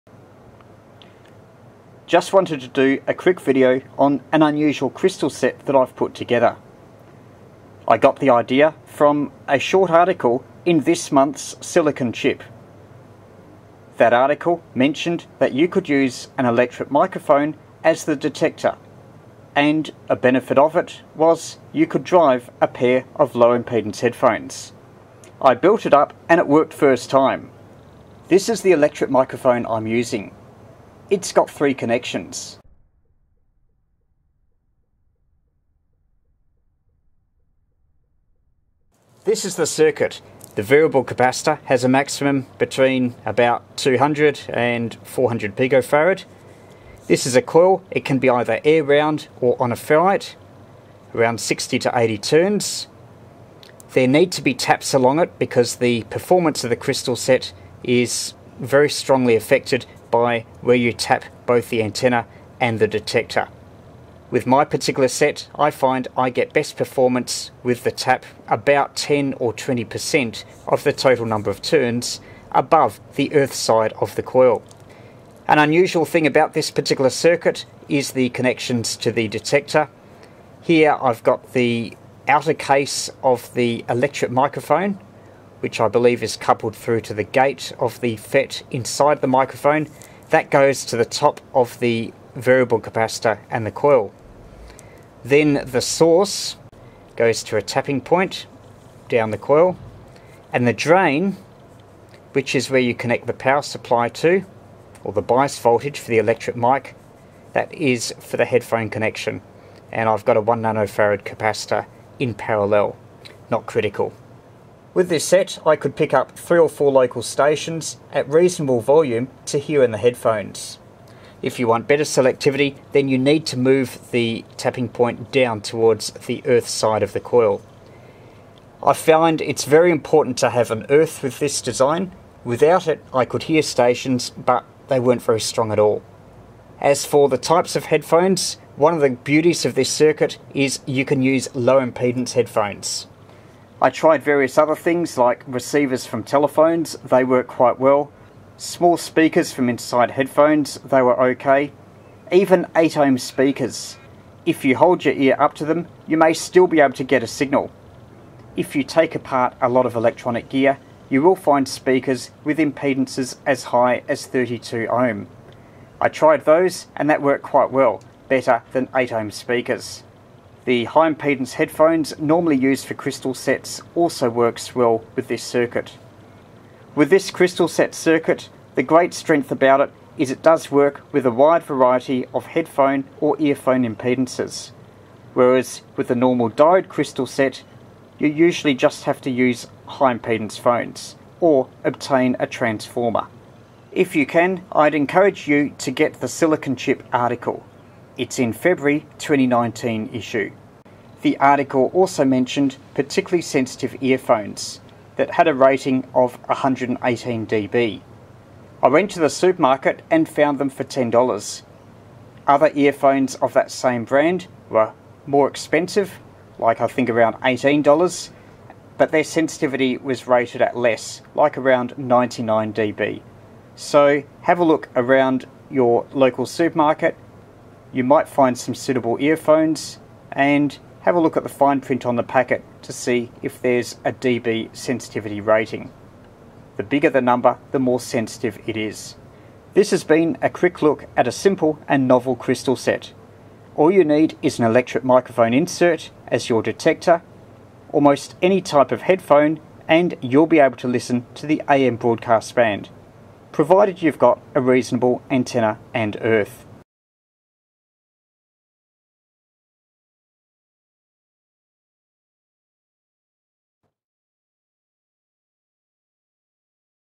An electret microphone crystal set
A simple crystal set capable of driving low impedance headphones. Inspired by article in February 2019 Silicon Chip magazine. Uses electret microphone as detector.